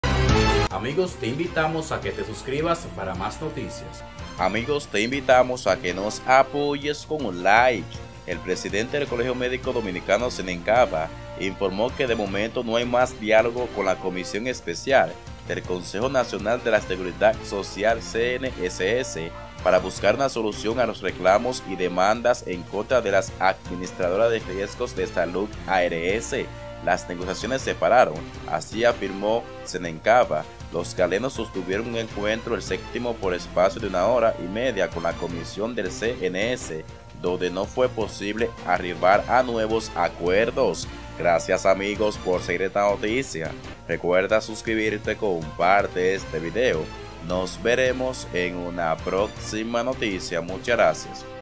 SENÉN CABA ROMPE EL DIÁLOGO CON EL CNSS: “LAS NEGOCIACIONES SE PARARON”
El presidente del Colegio Médico Dominicano (CMD), Senén Caba, informó este martes que, de momento, no hay más diálogo con la comisión especial del Consejo Nacional de la Seguridad Social (CNSS) para buscar una solución a los reclamos y demandas en contra de las Administradoras de Riesgos de Salud (ARS).

“Las negociaciones se pararon”, afirmó Caba en conversación con Diario Libre.

El lunes, los galenos sostuvieron un encuentro (el séptimo) por espacio de una hora y media con la comisión del CNSS donde no fue posible arribar a nuevos acuerdos. Desde la cuarta reunión, Caba había suavizado el tono de su discurso, asegurando que las conversaciones transitaban por caminos promisorios, lo que había elevado la esperanza entre los afiliados de que pronto se levantarían las suspensiones a las ARS seleccionadas.

Caba explicó que la decisión de no dialogar más se fundamenta en que el CNSS no quiere brindar respuesta inmediata al conflicto, y pide plazos de “tres y cuatro meses, cuando en menos de un año a las ARS les aumentaron 11 mil millones". "Todas las ofertas son futuristas”, indicó.